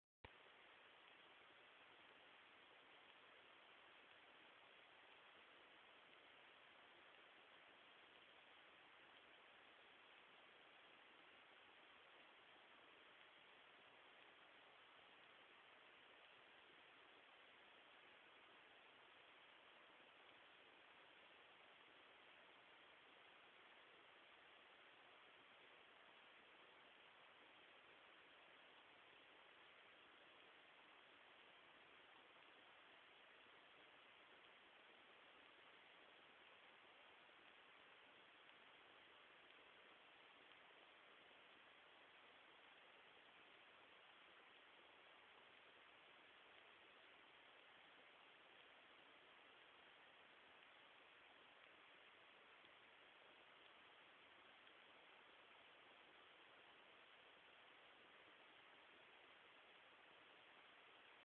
MASTERCAN Bulldog Yako obediencia antibocado
MASTERCAN Bulldog Yako obediencia antibocado - este perrito era muy tragon -  ya no se desespera x la comida